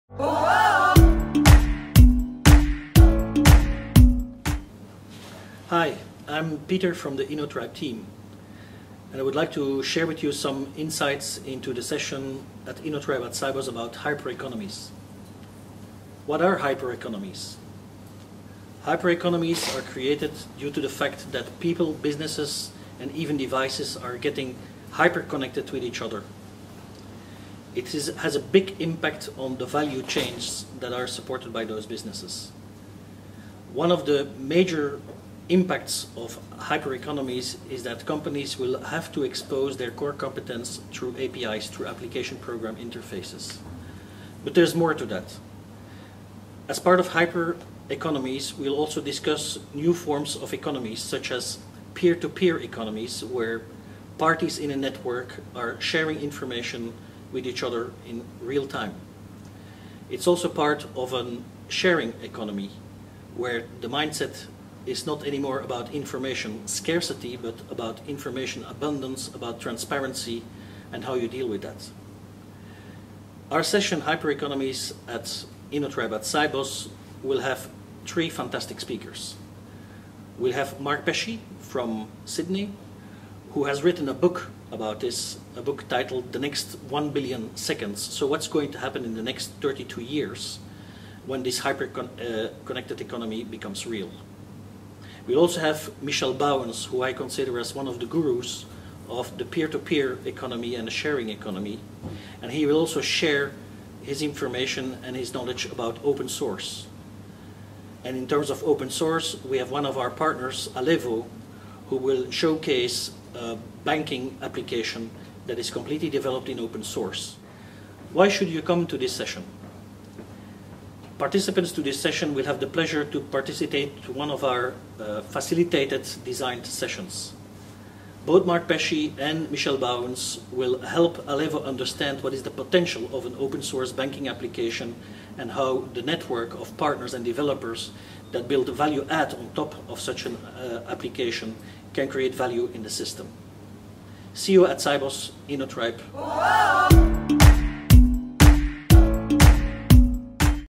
Innotribe@Sibos - Osaka 2012 - Hyper Economies session
Businesses, people, devices, so many things that are today inter connected. What does this mean for a company? What are the impacts on their business models and operational models? Should they care? What are the opportunities? Where can they get help from to deal with this? Listen to Peter Van describing the Hyper Economies session that will be available to you at Sibos Osaka this year in the Innotribe space.